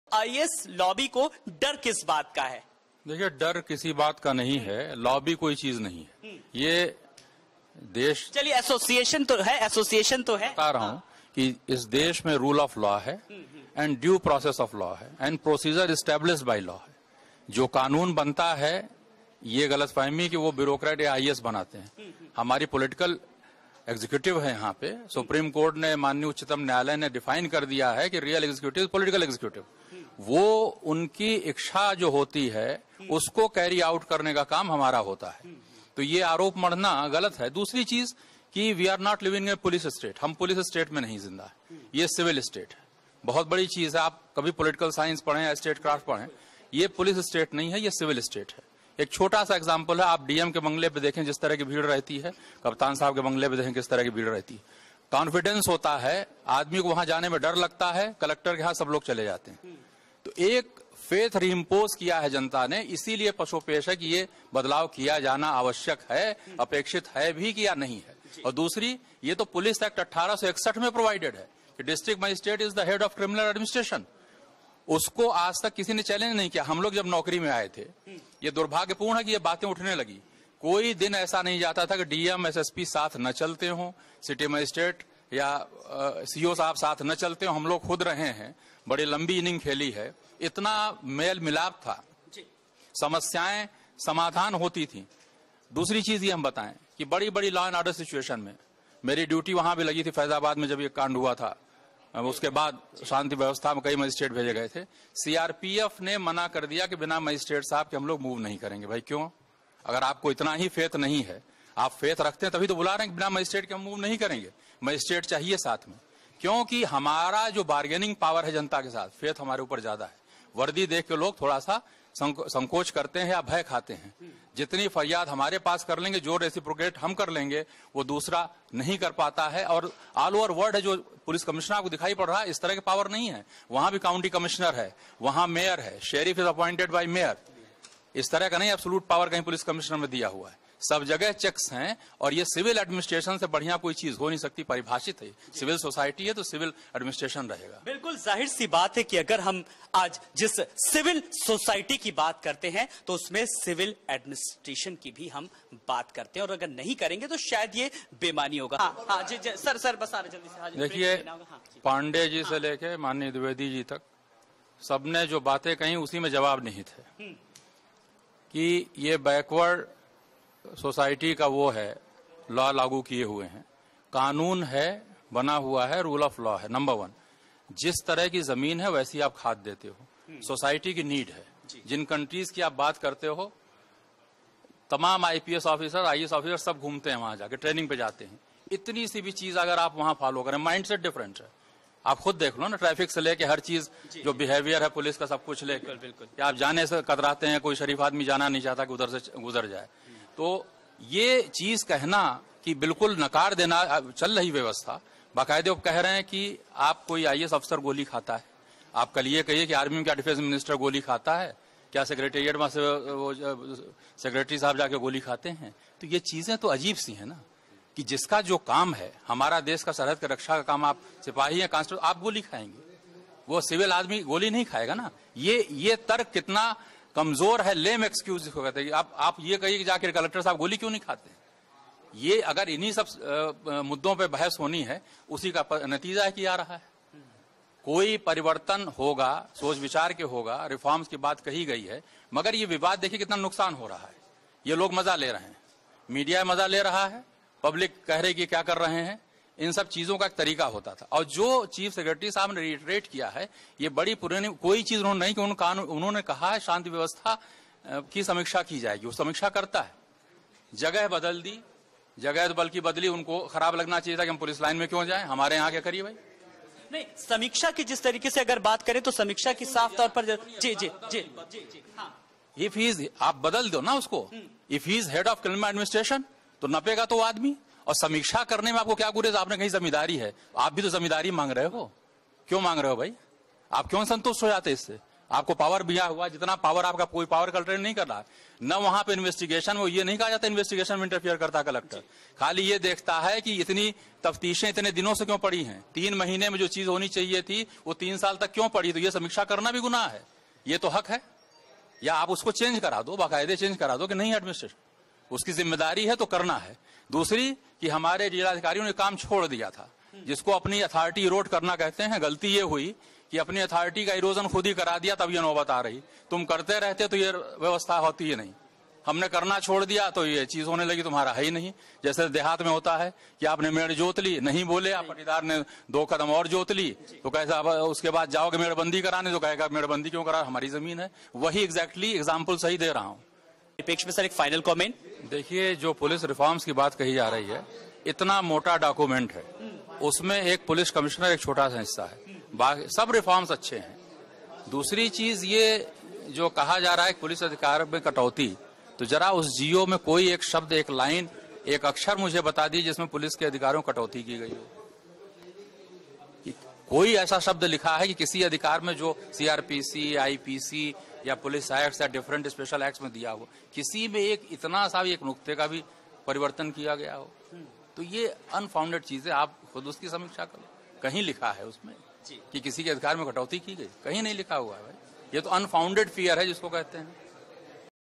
IAS Association stand on IAS vs IPS, DM-SSP relationship and Police Commissionerate system
Mr. Shri Prakash Singh, who initially started his career as PCS officer of 1979 batch and then got promoted as IAS officer of 1998 batch and retired as Secretary of Urban Development Department in Government of Uttar Pradesh explains IAS Association stand on IAS-IPS relationship especially at field level between DM and SSP/SP and views on Police Commissionerate system.